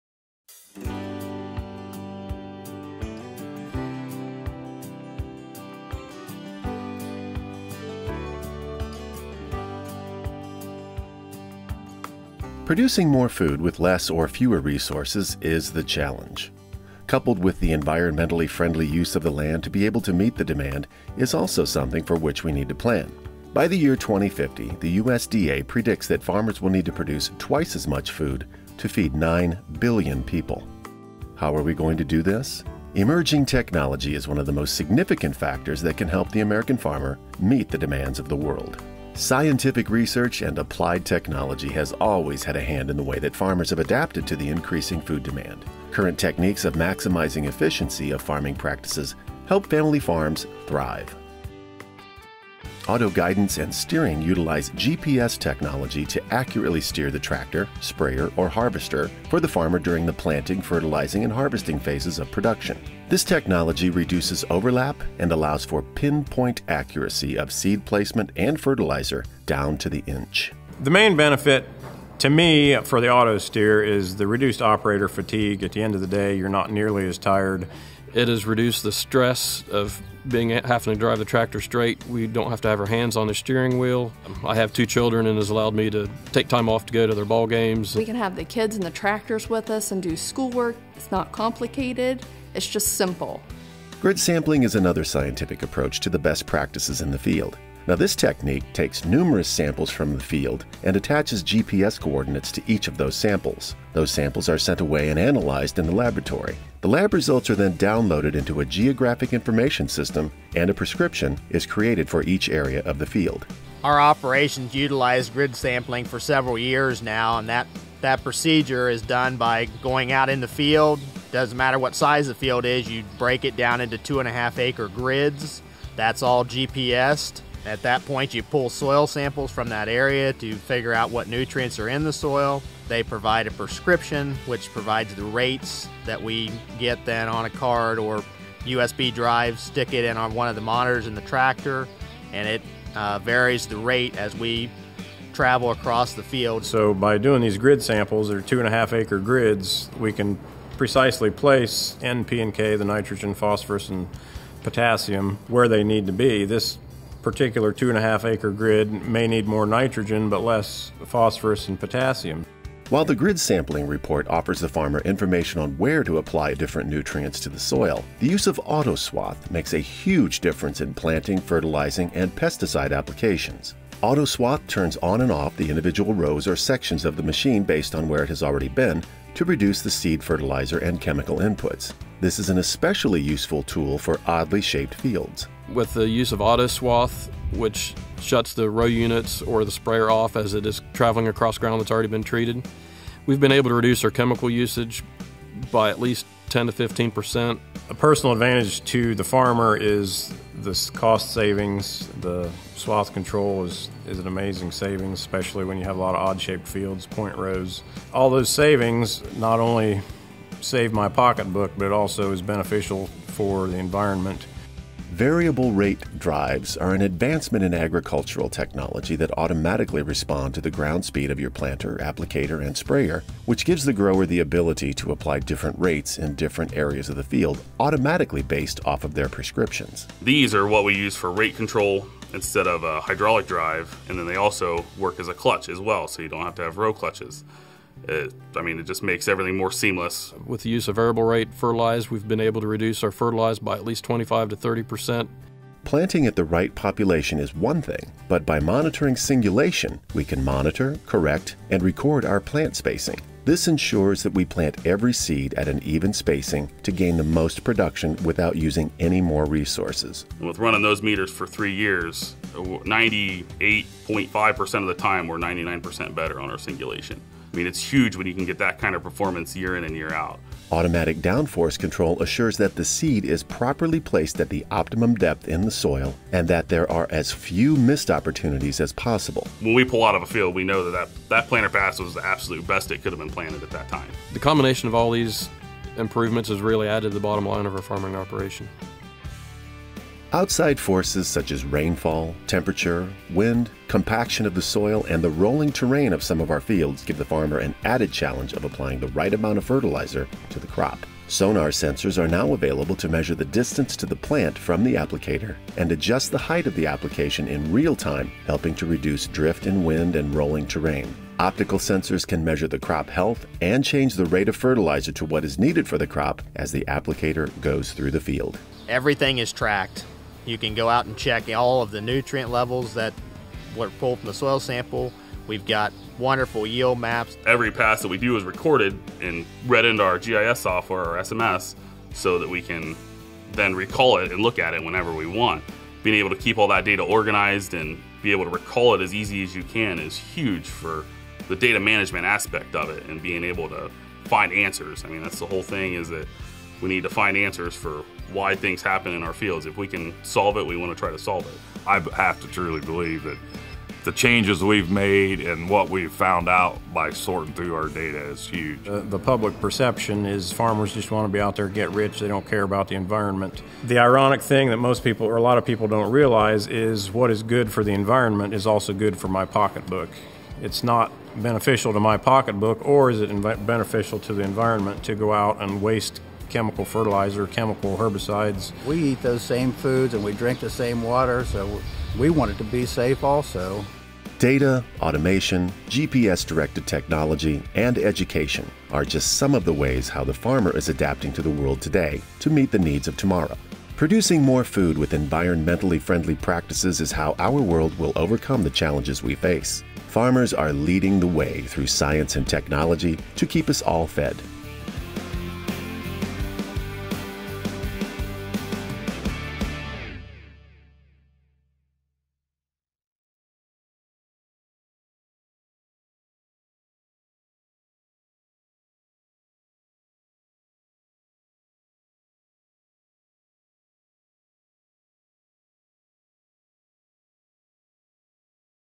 Family Farms Global Concerns - Technology in Agriculture
Feeding the World is the Challenge!  Ag Info Tech, LLC. created this unbranded video highlighting how the family farms that he deals with have implemented technology to not only help feed the world, but to reduce their environmental impact while producing more food.